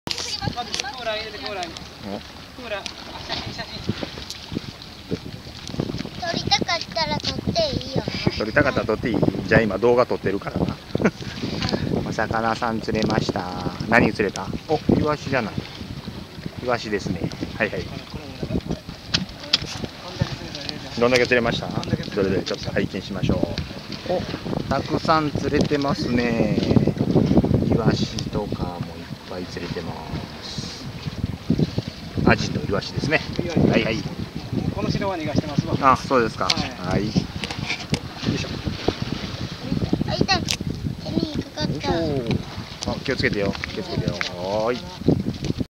fishingmax芦屋店『サビキ好調～♪』
豆アジやママカリ、イワシ等が狙えますよ♪